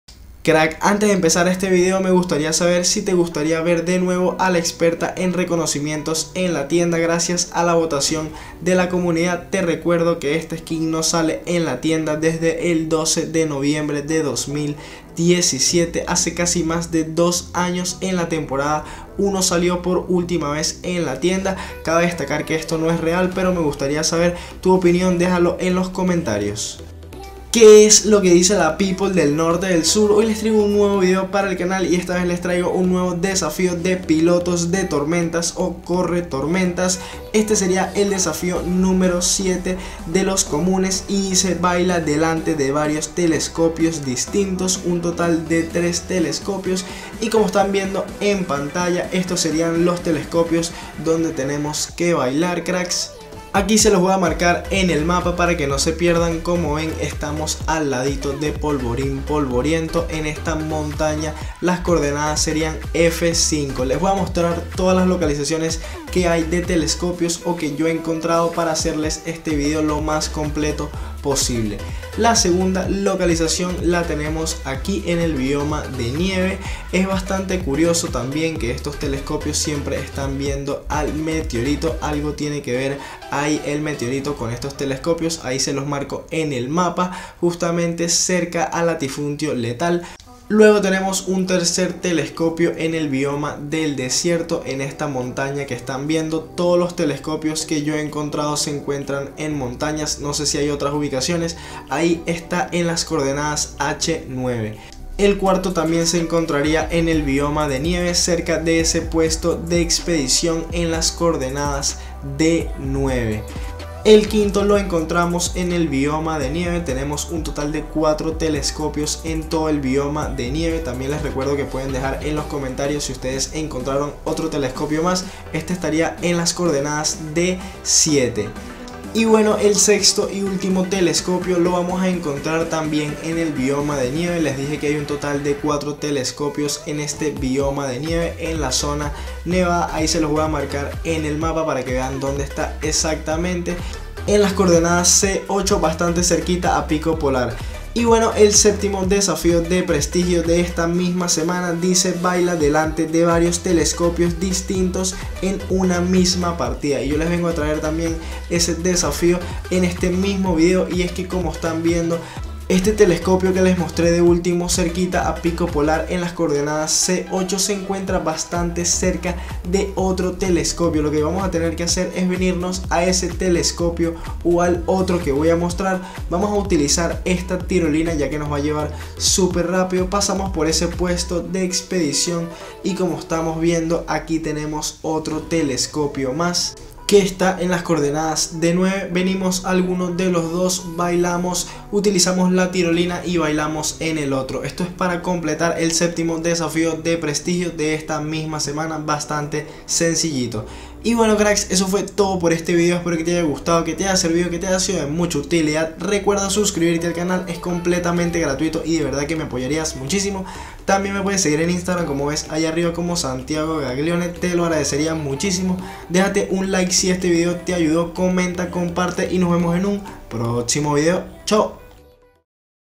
🔭 BAILA DELANTE DE VARIOS TELESCOPIOS DISTINTOS 🔭 DESAFIOS PILOTOS DE TORMENTAS/CORRETORMENTAS
⭐ BAILA DELANTE DE VARIOS TELESCOPIOS DISTINTOS / BAILA EN DISTINTOS TELESCOPIOS - DESAFÍOS SEMANA 9 TEMPORADA 10 (PILOTOS DE TORMENTAS/CORRETORMENTAS) ✅

Apóyame como Creador: SantiagoGaglione

La edición, clips y audio de este video fueron hechos por mi persona.